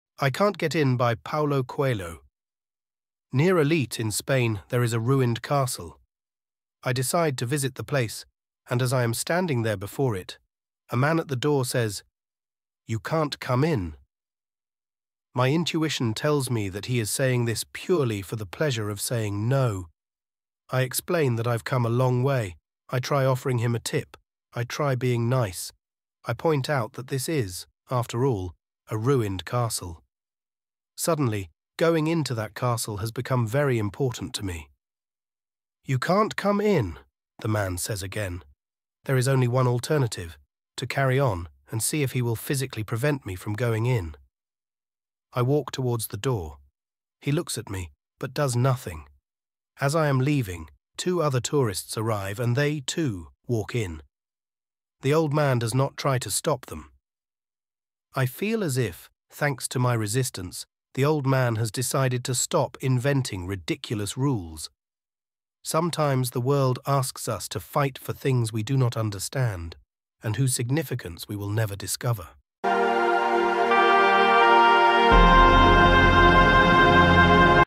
I Can't Get in | Coelho Coelho | English Story
Join us as we delve into an insightful story shared by the renowned author Paulo Coelho, titled "I Can't Get In." Set against the backdrop of a mesmerizing yet dilapidated castle near Olite, Spain, this narrative unfolds a simple yet profound encounter that transcends into a meaningful life lesson.

In this video, we explore Coelho's experience as he confronts an arbitrary denial of entry into the castle. Despite his genuine intentions and attempts to negotiate, he is repeatedly met with a steadfast "You can’t come in" from the gatekeeper. This encounter prompts a deeper reflection on the essence of persistence and the subtle art of challenging unwarranted barriers.